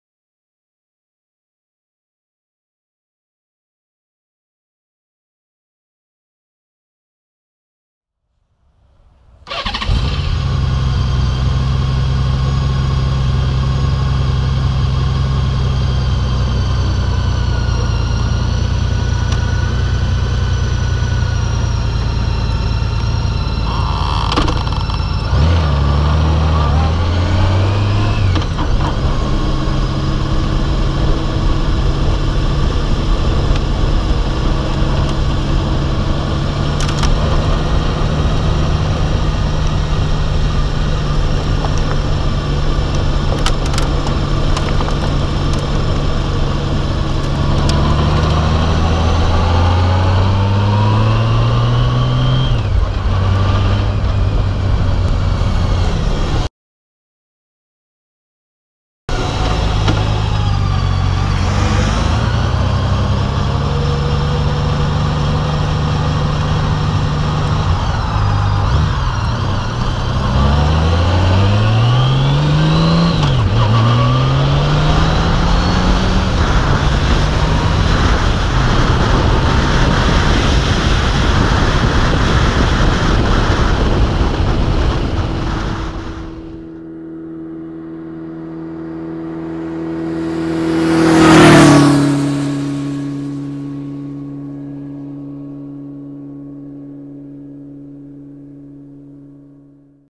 2014 Honda Valkyrie
car pulling out in front of me caught on GoPro